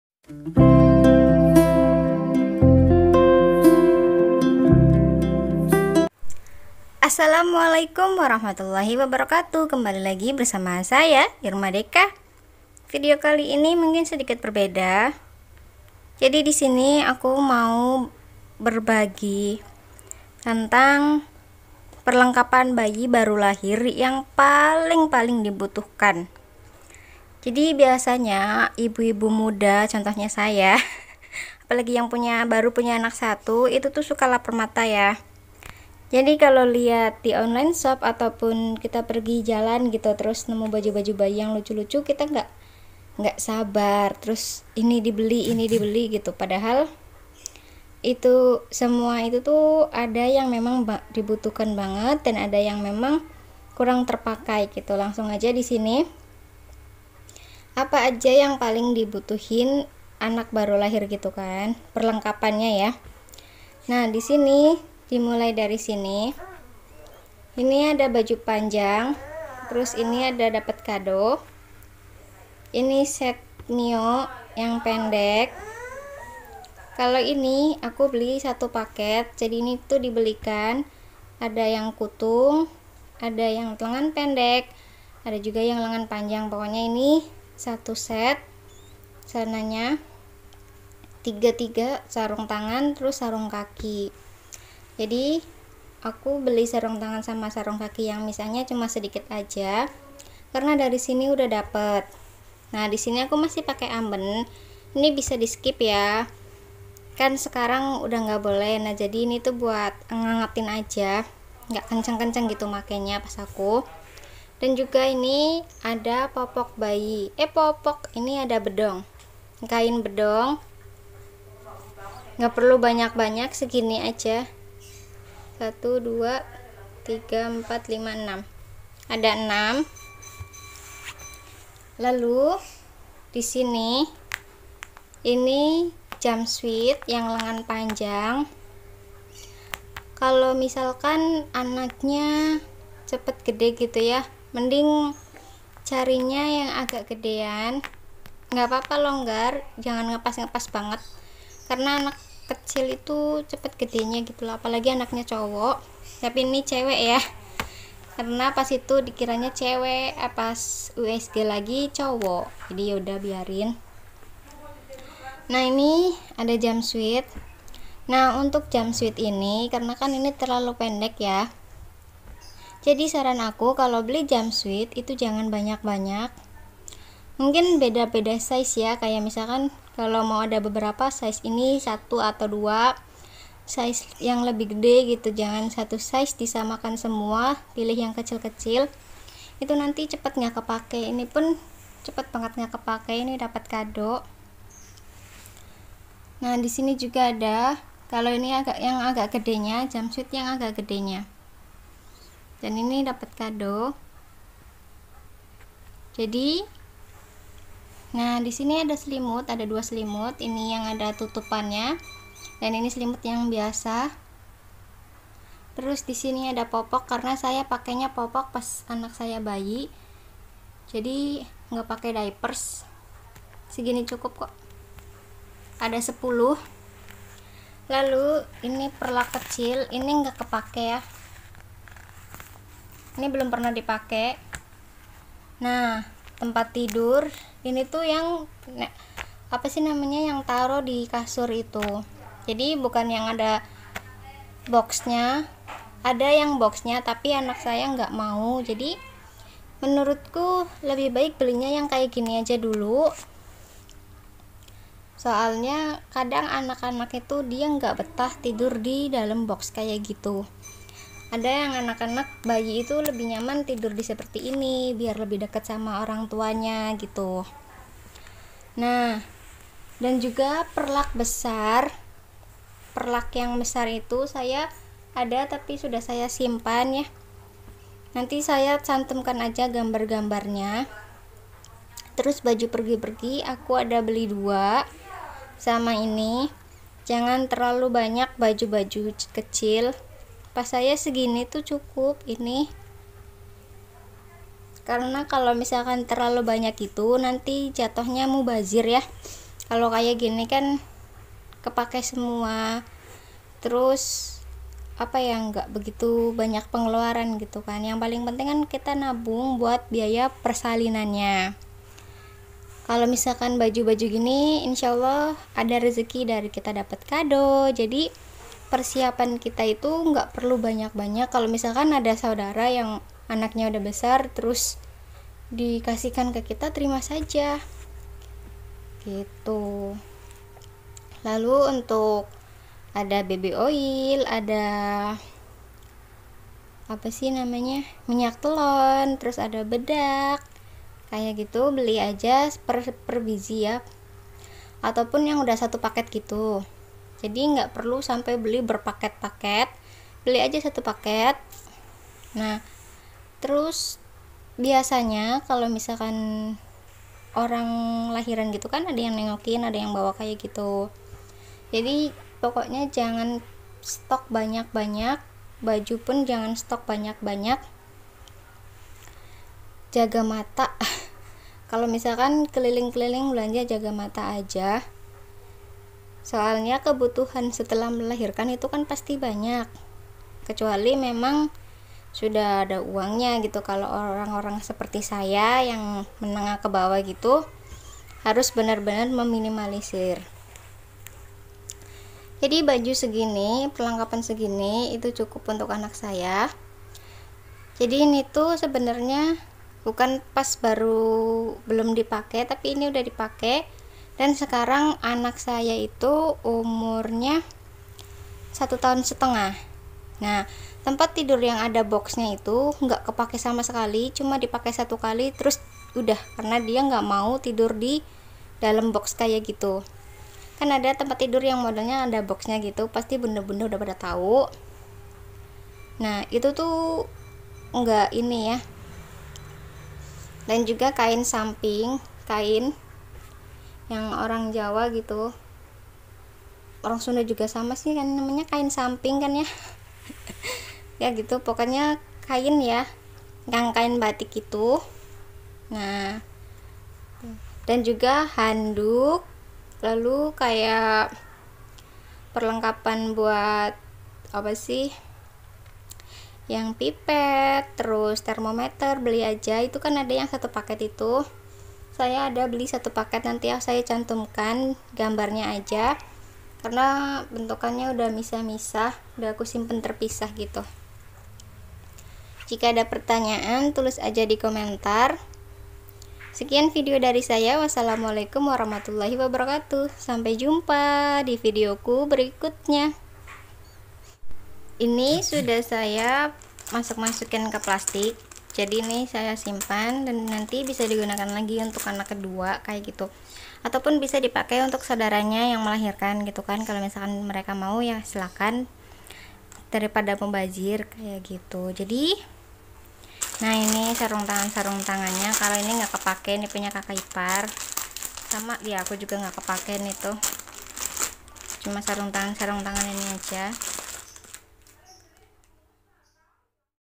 Perlengkapan bayi baru lahir yang paling dibutuhkan | anti mubazir
Assalamualaikum..
jadi divideo ini aku hanya share pakaian aja dan lain2nya yg dibutuhkan.

yg paling penting adalah dana untuk biaya persalinan dan juga biaya setelah melahirkan.